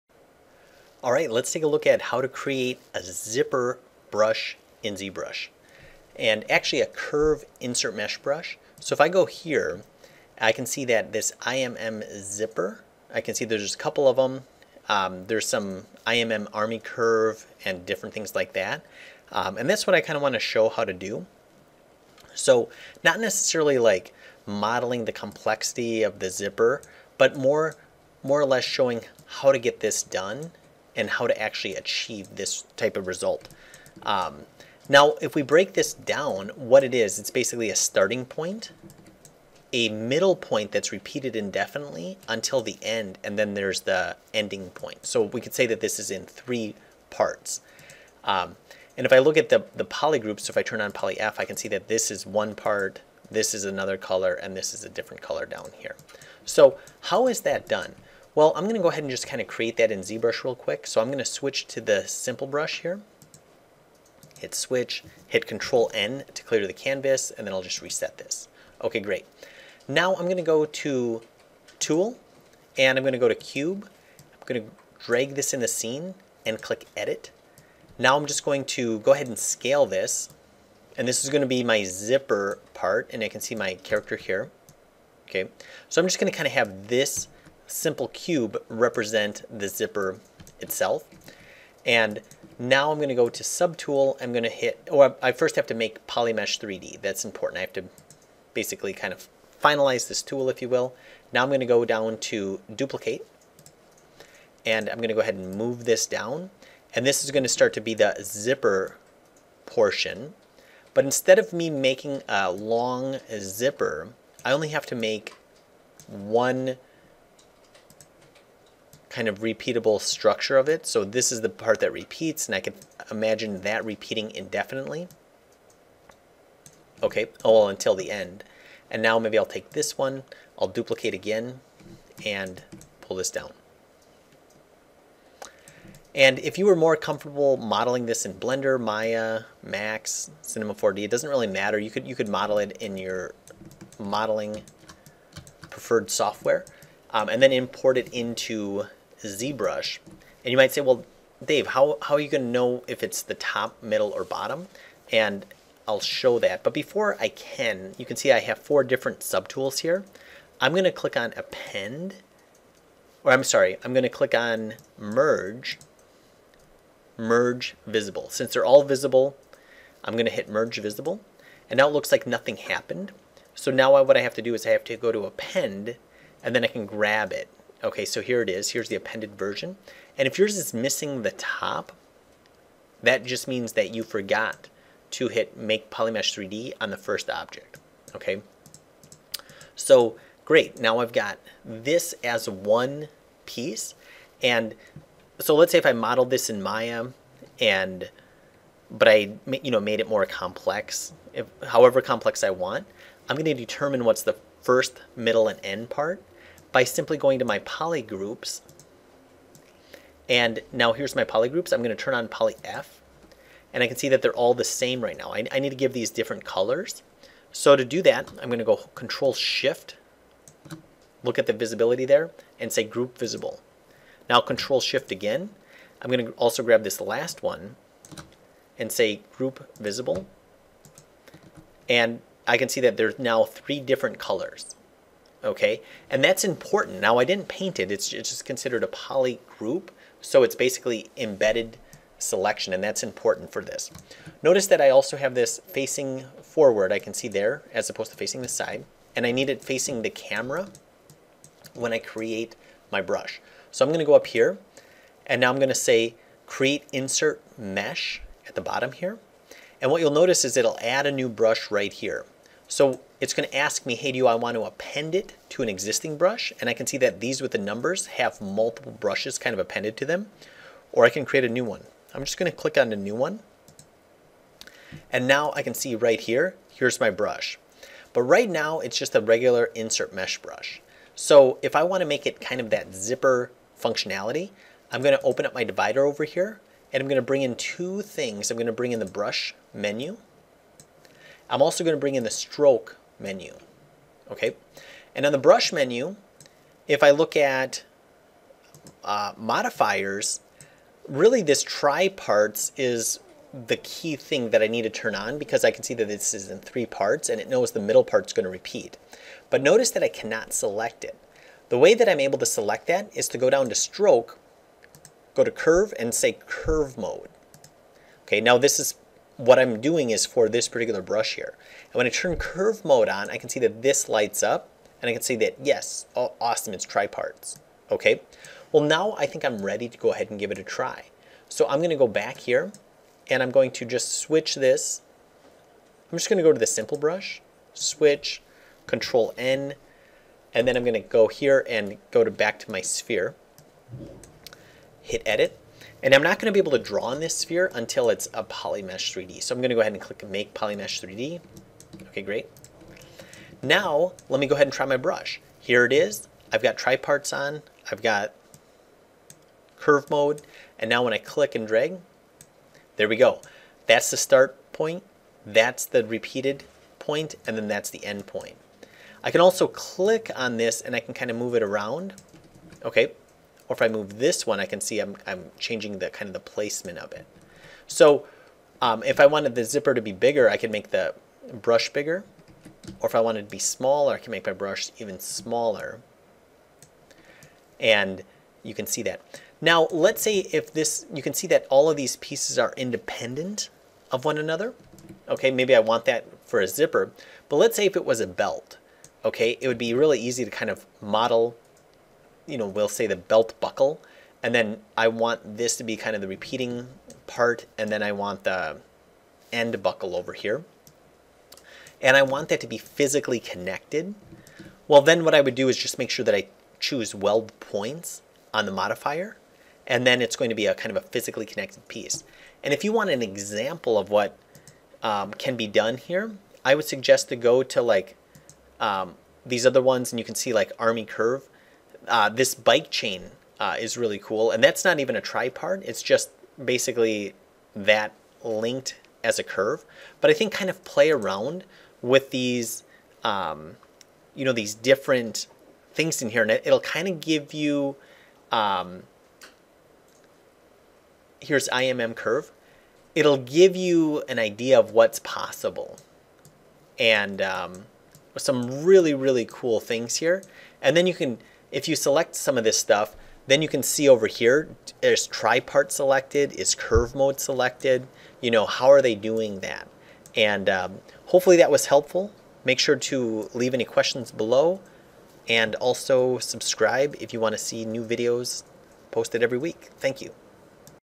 Custom Zipper in ZBrush (Creating own IMM Curve)
Learn how to create a custom zipper in ZBrush in this quick tutorial! Creating custom brushes is a great way to speed up your workflow and add unique details to your 3D models. In this tutorial, you'll learn how to create your own IMM curve brush inside of ZBrush to help bring your creations to the next level. We'll cover all the steps needed to create a custom zipper brush and show you how to use it to add zippers to your models. Whether you're new to ZBrush or looking to expand your skills, this tutorial is a must-see!

TIMECODES:
0:00        Intro
1:11        Create IMM Brush
2:41               Merging Multiple Subtools
3:27               Setting up Polygroups
4:13               Create InsertMesh
4:49               Setting up Curve Options
5:44        Testing out Curve Brush